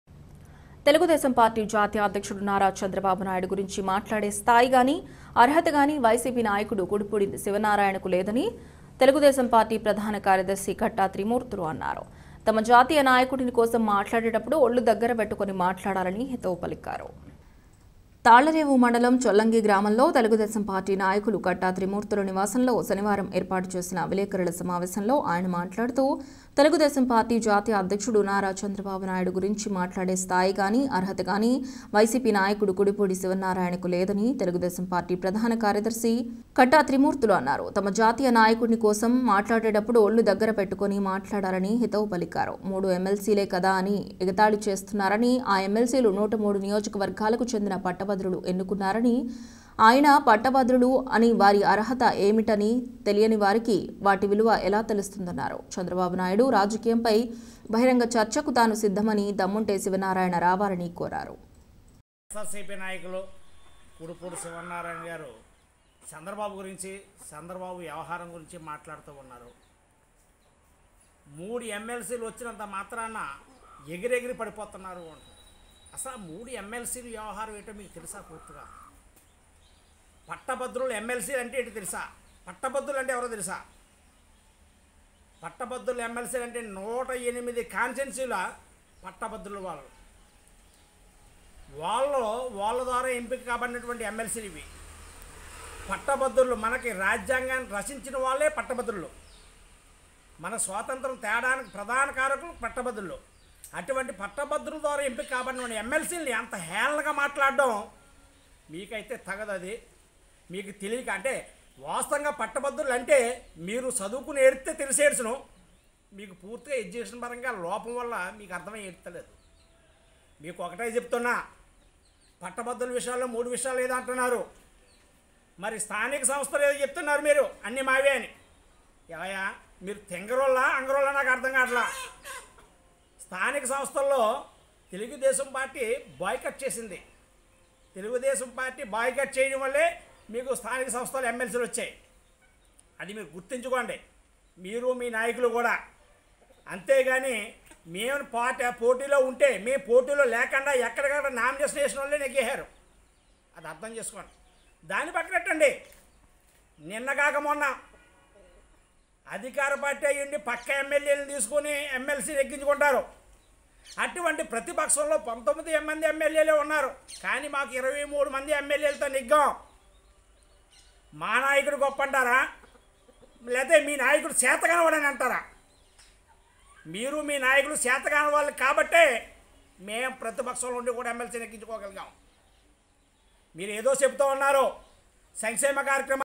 చంద్రబాబు నాయుడు గురించి మాట్లాడే స్ధాయి..గాని అర్హత గాని వైసీపీ నాయకులకు లేదన్న కట్టా త్రిమూర్తులు
తెలుగు దేశం పార్టి జాతీయ అధ్యక్షుడు నారా చంద్రబాబు నాయుడు గురించి మాట్లాడే స్ధాయి..గాని అర్హత గాని  వైసీపీ నాయకుడు కుడిపూడి శివన్నారాయణ కు లేదని తెలుగుదేశం పార్టీ ప్రదాన కార్రదర్శీ కట్టా త్రిమూర్తులు అన్నారు. తమ జాతీయ నాయకుని కోసం మాట్లాడేటప్పుడు ఒళ్ళుదగ్గర పెట్టుకు నిమాట్లాడాలని హిథవుపలికారు...